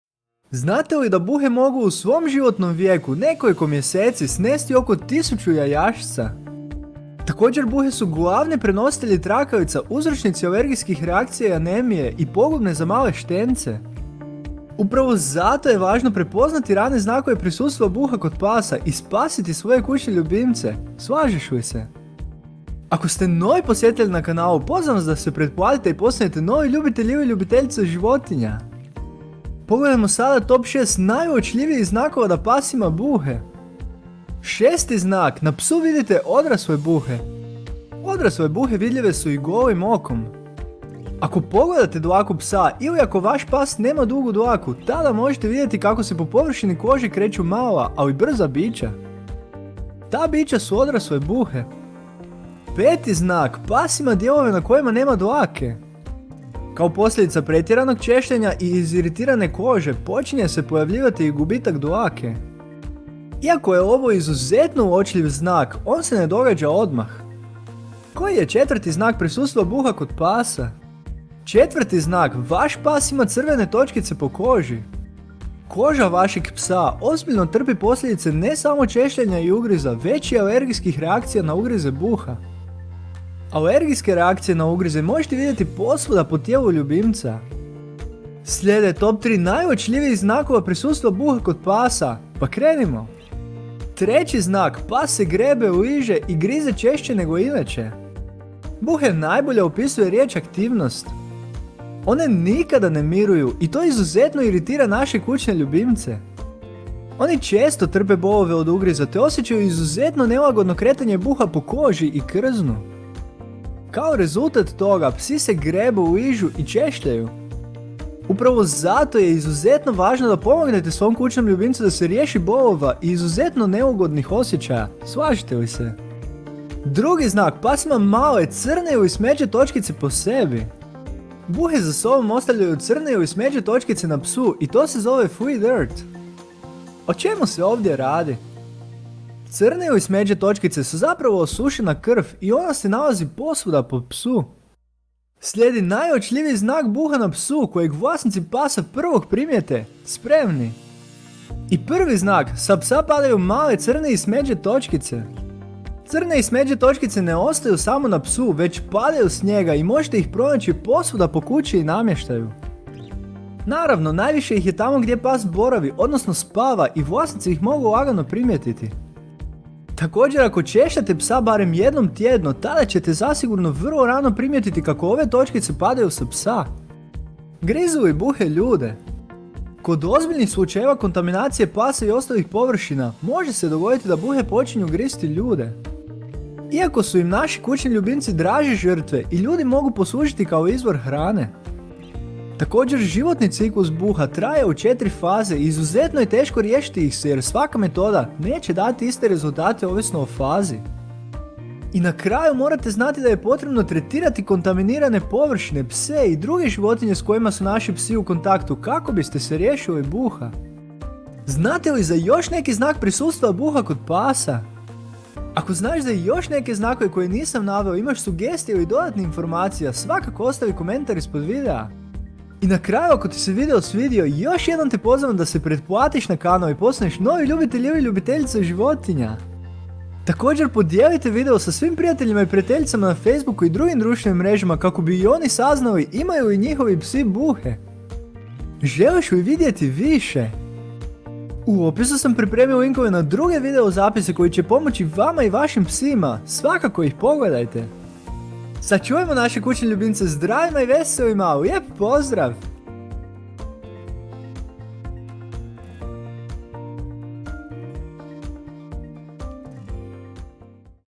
🐕 TOP 6 Najuočljivijih Znakova Da Pas Ima Buhe!
👉 Lista TOP 6 najuočljivijih znakova da pas ima buhe otkriva vam prve i najvažnije simptome prisustva iritirajućih i za neke pse opasnih buha...Koji su to znakovi?
⬇️ Više ⬇️
Posjeti moj webshop KUĆNI LJUBIMCI SHOP na adresi:

5% POPUSTA ❤️❤️❤️ uz kupon "LJUBIMCI"

Znate li da buhe mogu u svom životnom vijeku (nekoliko mjeseci) snesti oko 1000 jajašca.

Također buhe su:
• glavni prenositelji trakavica,
• uzročnici alergijskih reakcija i anemije,
• pogubne za male štence.

Upravo zato je važno znati prepoznati rane znakove prisustva buha kod pasa i spasiti svoje kućne ljubimce, slažeš li se?

Pogledajmo sada top 6 najuočljivijih znakova da pas ima buhe:

6. Na Psu Vidite Odrasle Buhe.

Odrasle buhe vidljive su i golim okom.

Ako pregledate dlaku psa ili ako vaš pas nema dugu dlaku, tada možete vidjeti kako se po površini kože kreću mala, ali brza bića.

Ta bića su odrasle buhe.

5. Pas Ima Dijelove Na Kojima Nema Dlake.

Kao posljedica pretjeranog češljanja i iziritirane kože počinje se pojavljivati i gubitak dlake.

Iako je ovo izuzetno uočljiv znak, on se ne događa odmah.

Koji je 4 znak prisustva buha kod pasa?

4. Vaš Pas Ima Crvene Točkice Po Koži.

Koža vašeg psa ozbiljno trpi posljedice ne samo češljanja i ugriza, već i alergijskih reakcija na ugrize buha.

Alergijske reakcije na ugrize možete vidjeti posvuda po tijelu ljubimca.

Slijede top 3 najuočljivijih znakova prisustva buha kod pasa, pa krenimo!

3. Pas Se Grebe, Liže I Grize Češće Nego Inače.

Buhe najbolje opisuje riječ aktivnost.

One nikada ne miruju i to izuzetno iritira naše kućne ljubimce.

Oni često trpe bolove od ugriza, te osjećaju izuzetno neugodno kretanje buha po koži i krznu.

Kao rezultat toga psi se:
• grebu,
• ližu i
• češljaju.

Upravo zato je izuzetno važno da pomognete svome kućnom ljubimcu da se riješi bolova i neugodnih osjećaja, slažete li se?

2. Pas Ima Male Crne Ili Smeđe Točkice Po Sebi.

Buhe za sobom ostavljaju crne ili smeđe točkice na psu i to se zove „flea dirt“.

O čemu se ovdje radi?

Crne ili smeđe točkice su zapravo osušena krv i ona se nalazi posvuda po psu.

Slijedi najuočljiviji znak buha na psu kojeg vlasnici pasa prvog primijete, spremni?

1. Sa Psa Padaju Male Crne i Smeđe Točkice.

Crne i smeđe točkice ne ostaju samo na psu već padaju s njega i možete ih pronaći posvuda po kući i namještaju.

Naravno najviše ih je tamo gdje pas boravi odnosno spava i vlasnici ih mogu lagano primjetiti.

Također, ako češljate svog psa barem jednom tjedno tada ćete zasigurno vrlo rano primijetiti kako ove točkice padaju sa psa.

Grizu li buhe ljude?

Kod ozbiljnijih slučajeva kontaminacije pasa i ostalih površina može se dogoditi da buhe počinju gristi ljude.

Iako su im naš kućni ljubimci draže žrtve i ljudi im mogu poslužiti kao izvor hrane.

Također životni ciklus buha traje u 4 faza i izuzetno je teško riješiti ih se jer svaka metoda neće dati iste rezultate.

I na kraju morate znate da je potrebno tretirati kontaminirane površine, pse i druge životinje s kojima su naši psi u kontaktu kako biste se riješili buha.

Želite li vidjeti više?

Materijali u videu:
1. Buhe Na Psu: Pogledajte 7 Načina Kako Se Riješiti Buha Na Psu Odmah: